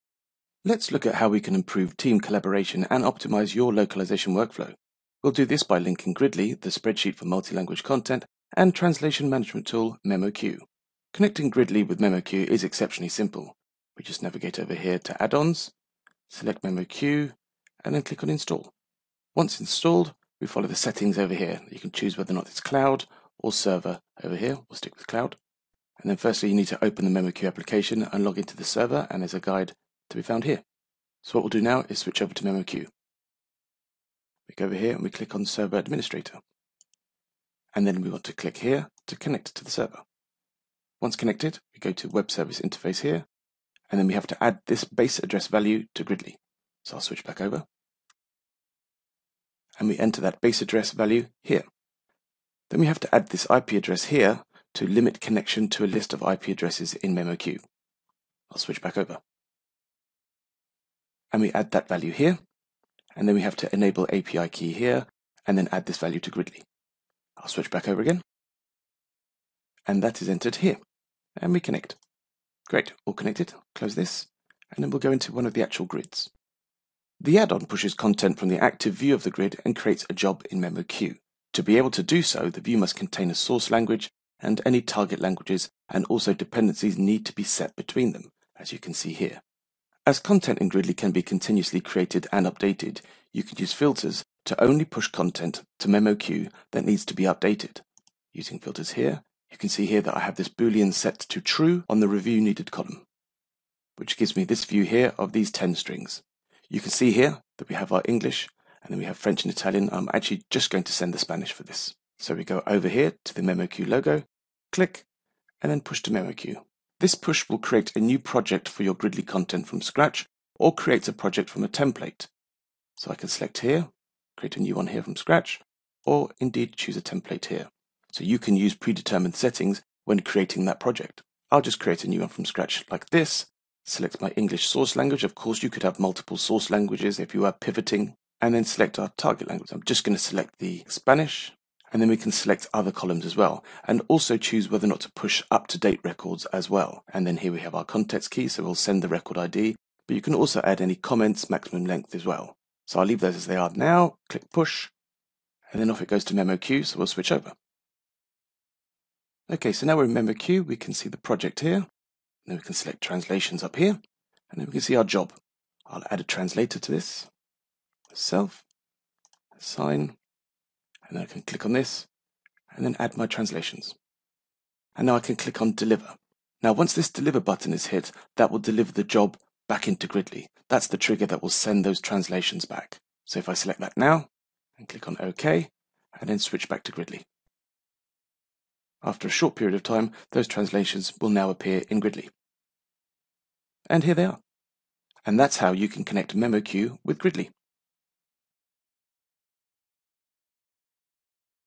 Connect memoQ with Gridly: out-of-the-box integration
Streamline your translation processes by connecting Gridly with the leading collaborative TMS: memoQ